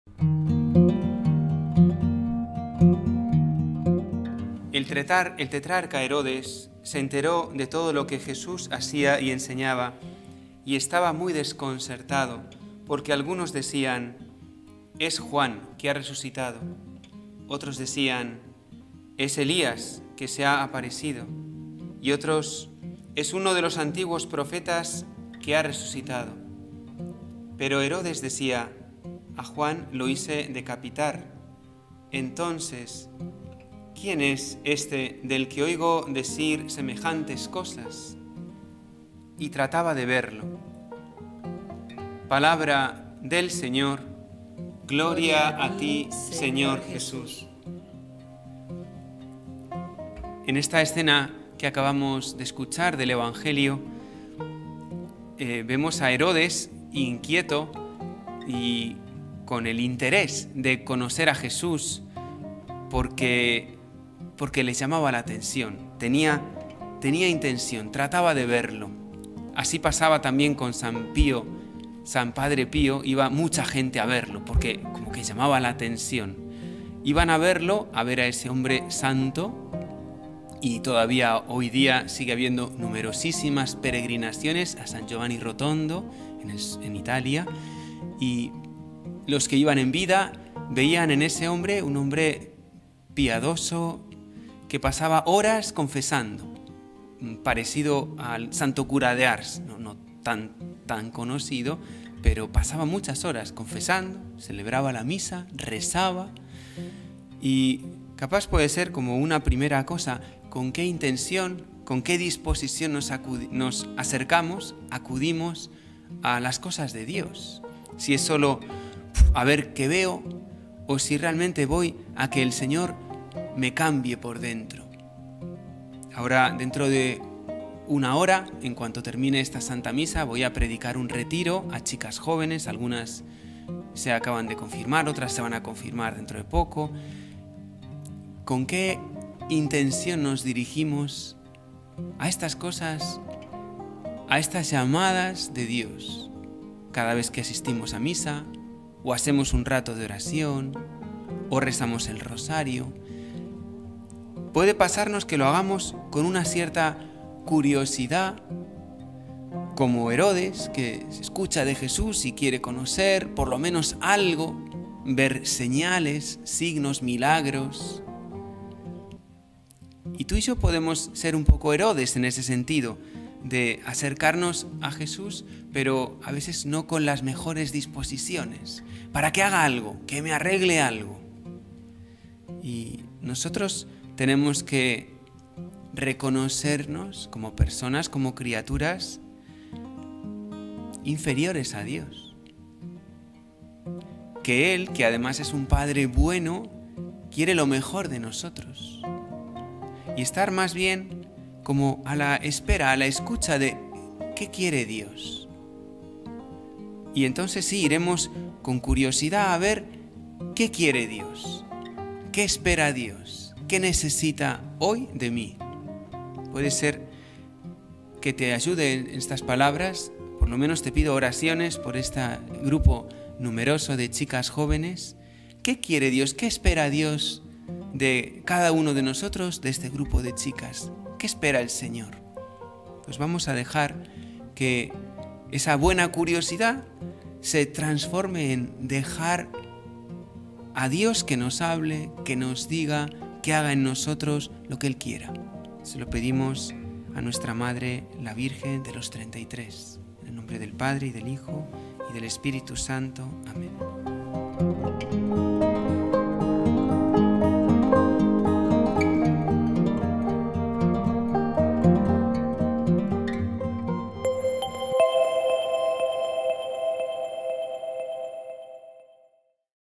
Evangelio del día - Lucas 9, 7-9 | 23 de Setiembre 2021
Te compartimos el Evangelio y la homilía del Jueves 23 de Setiembre del 2021 a cargo del  P. Elías Capapé

    El tetrarca Herodes se enteró de todo lo que pasaba, y estaba muy desconcertado porque algunos decían: «Es Juan, que ha resucitado.» Otros decían: «Es Elías, que se ha aparecido», y otros: «Es uno de los antiguos profetas que ha resucitado.»
    Pero Herodes decía: «A Juan lo hice decapitar. Entonces, ¿quién es este del que oigo decir semejantes cosas?» Y trataba de verlo.